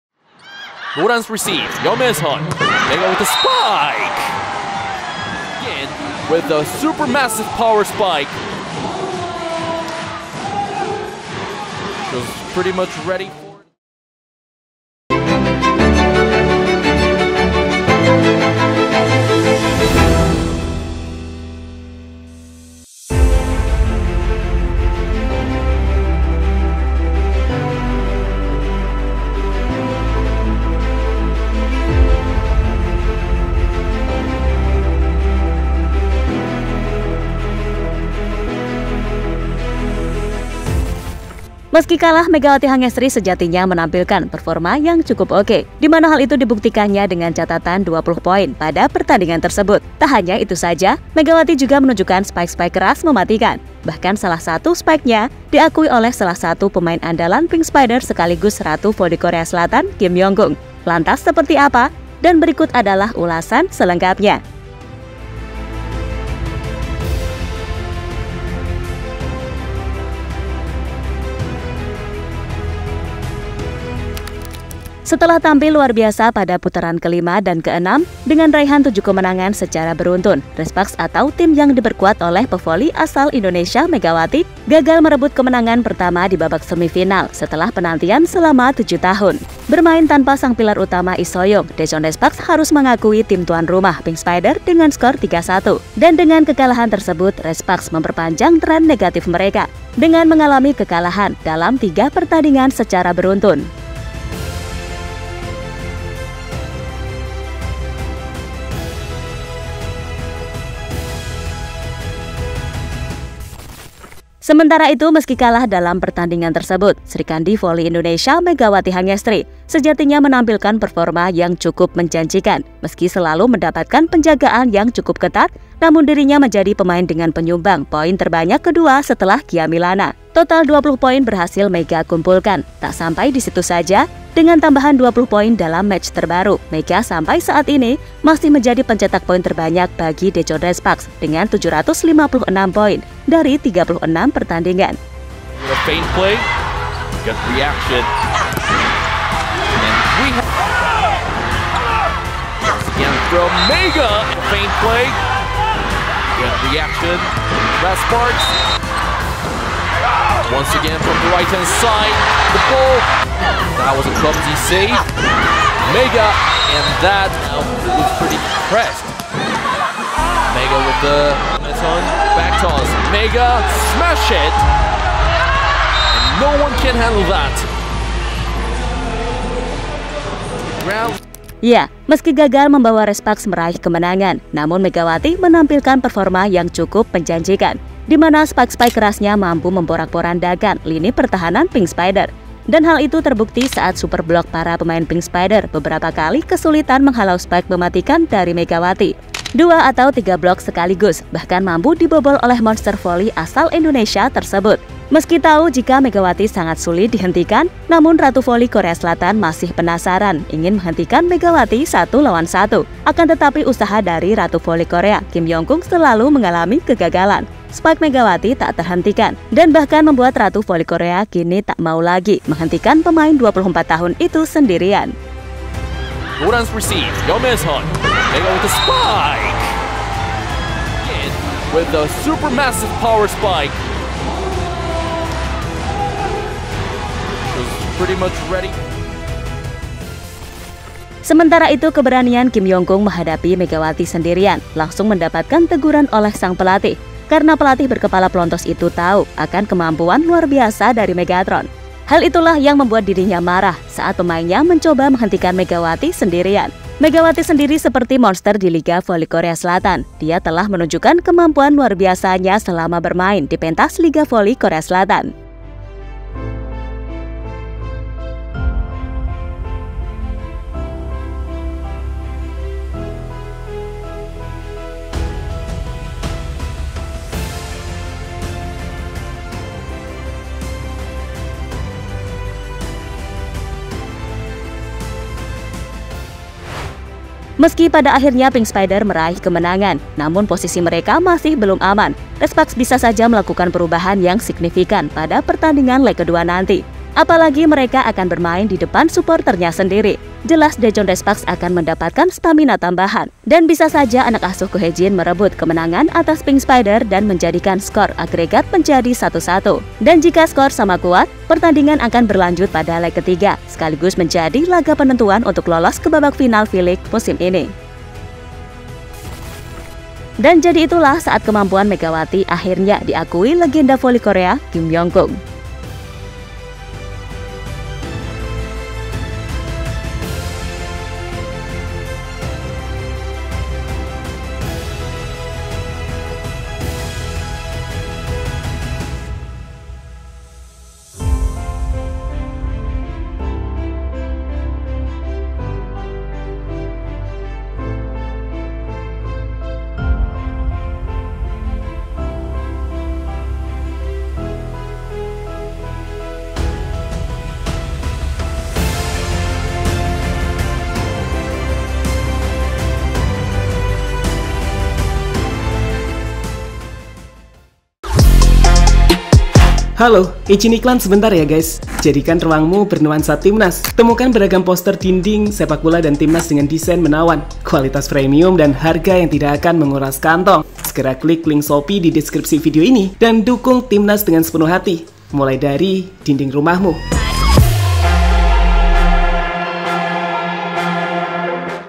Masih Penasaran Duel 1 vs 1 Dengan Mega!!KYK Akhirnya Mengaku Menyerah Tak Mau Hentikan Sendirian
Kunjungi toko kami di Shopee!

Masih Penasaran Duel 1 vs 1 Dengan Mega !! KYK Akhirnya Mengaku Menyerah Tak Mau Hentikan Mega Sendirian